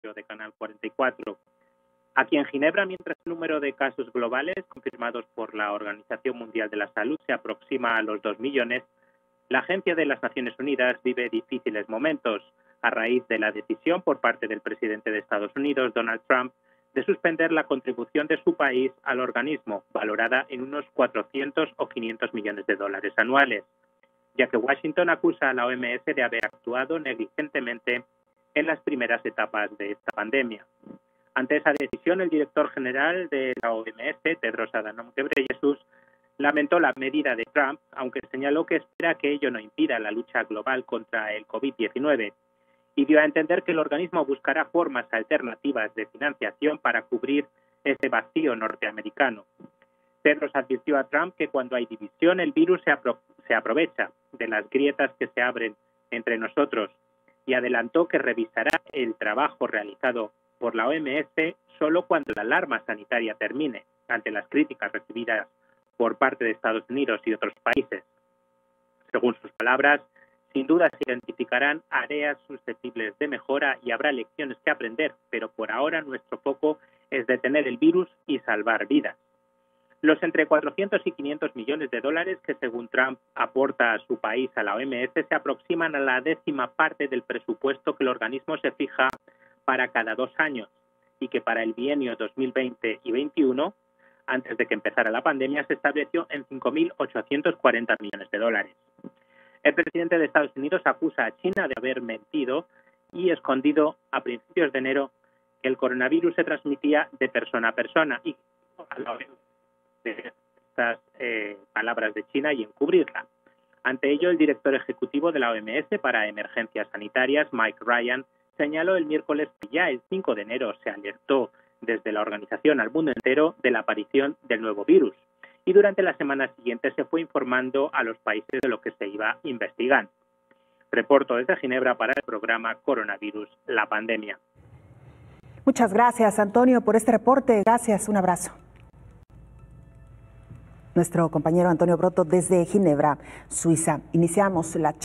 OMS vive momentos difíciles después del retiro del financiamiento de Estados Unidos
Tedros Adhanom Ghebreyesus Director General de la OMS adelantó que revisará el trabajo realizado por la organización cuando ceda la crisis pues por ahora el interés está en salvar vidas.

Un reporte de Antonio Broto de EFE noticias desde Ginebra, Suiza